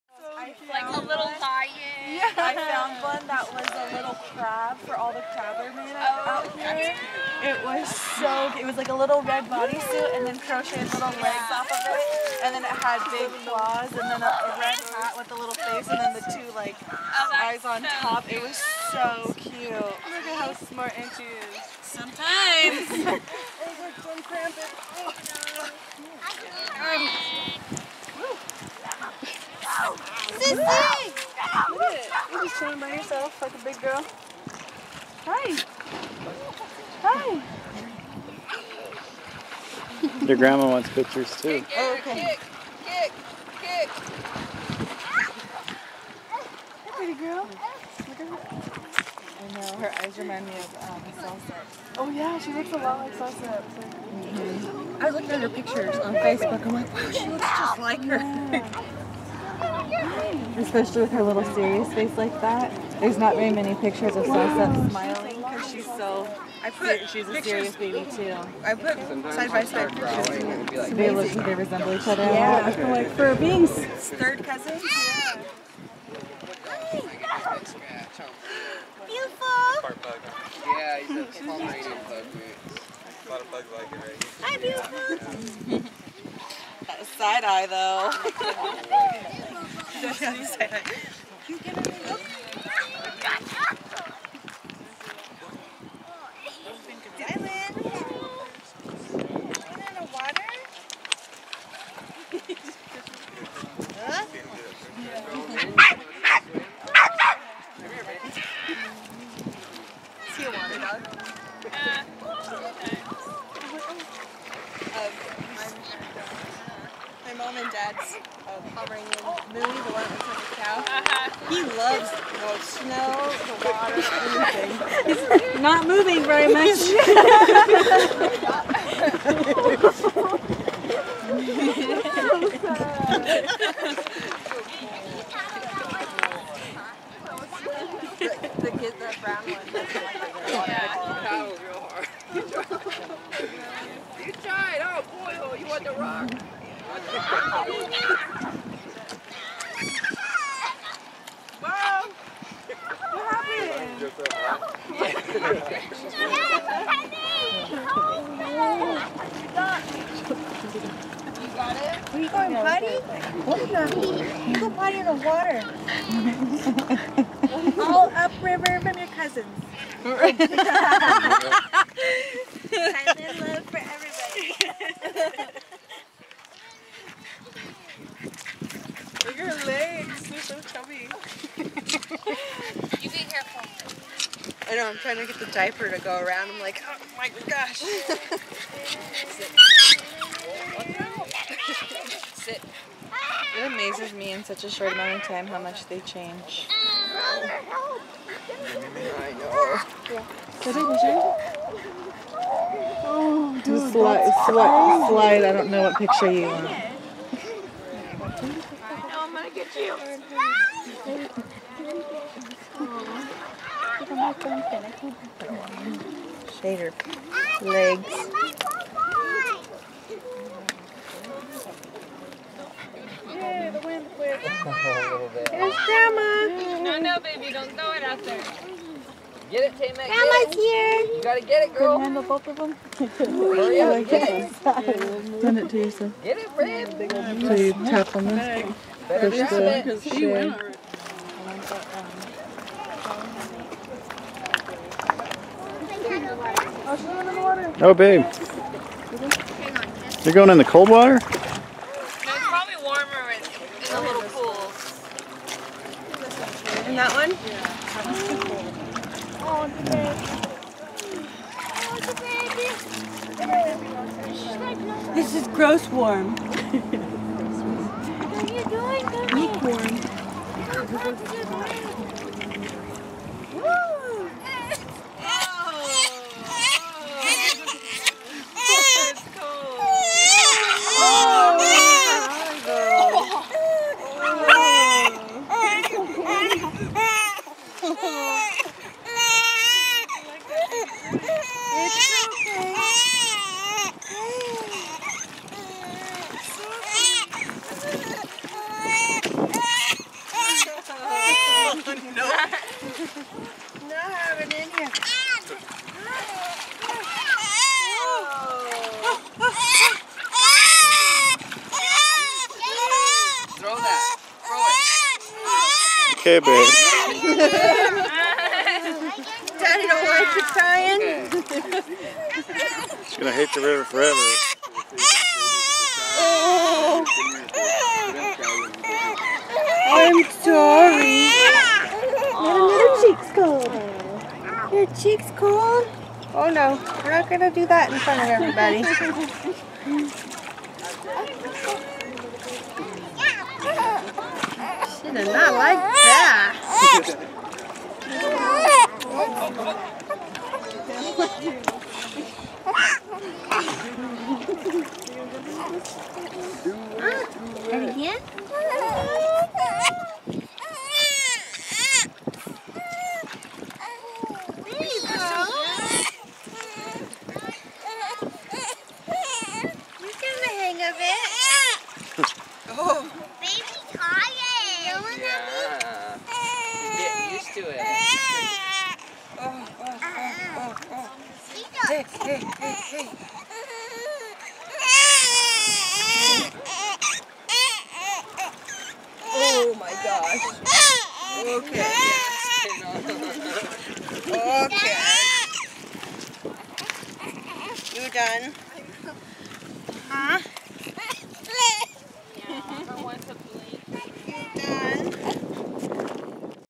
Zylynn's First River Trip
Zylynn's first trip to the Smith River. She didn't like the cold water at all.